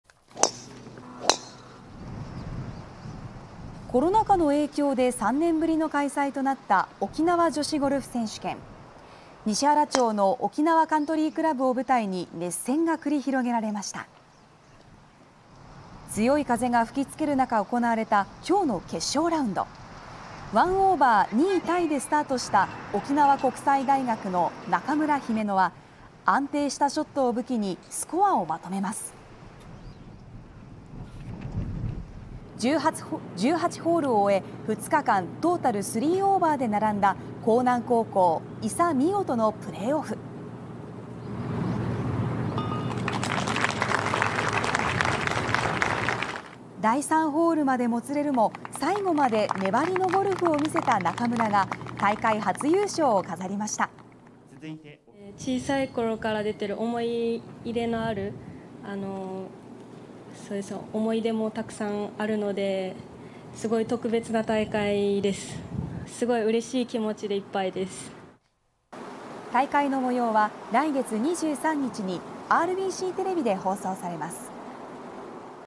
女子ゴルフ選手権 3年ぶりに開催
沖縄女子プロゴルフの登竜門・沖縄女子ゴルフ選手権が3年ぶりに開催されました。2022/10/27